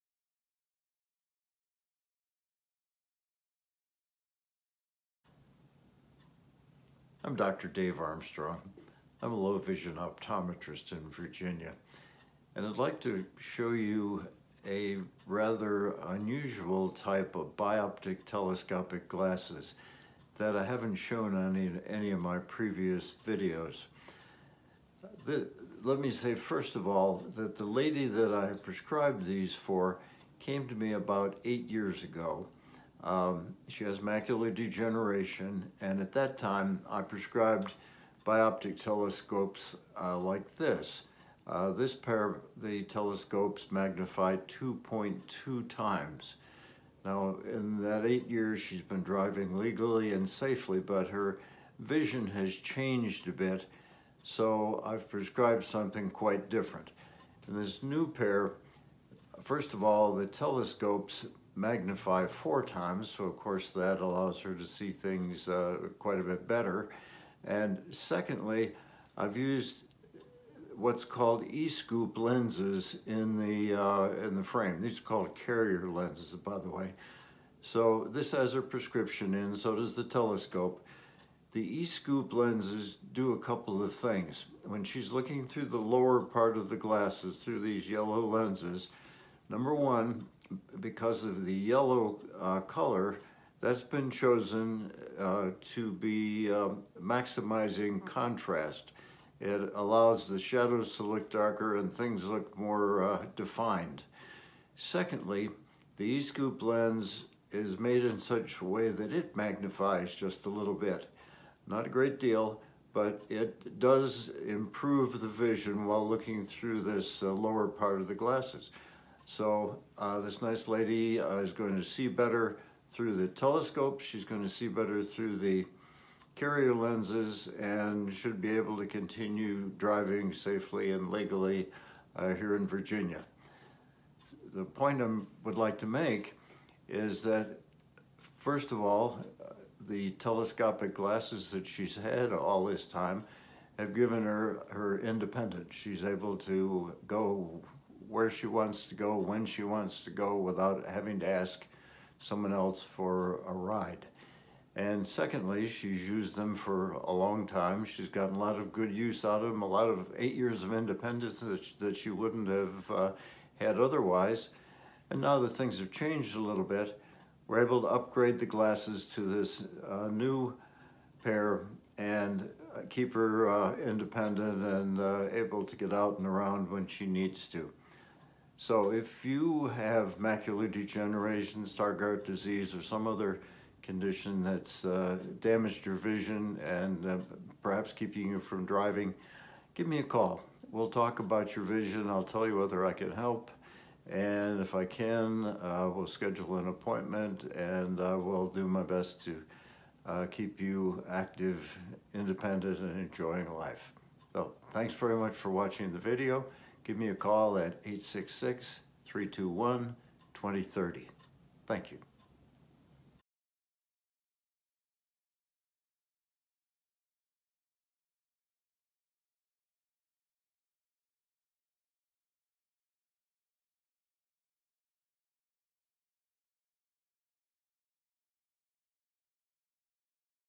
New Bioptic Glasses Help a Lady With Macular Degeneration Keep Driving
One of my nice patients, a nice 84 year old lady, has been driving while wearing bioptic telescopic glasses for 8 years.  That's very important to her because she's able to remain independent.  She lives alone and without the glasses she thinks she'd have to give up her home and move to an assisted living facility, something she does not want to do.
I recently made some significant changes in her glasses and she noticed immediate improvement in her vision.  She was able to see more sharply and sees the details of distant objects better.  They make driving easier and safer for her.
In the video I describe the new glasses.
People with macular degeneration often have to give up driving and become dependent on other people to take them places.  They lose much of their independence.  I have helped many people with macular degeneration continue driving by wearing bioptic telescopic glasses that I've prescribed.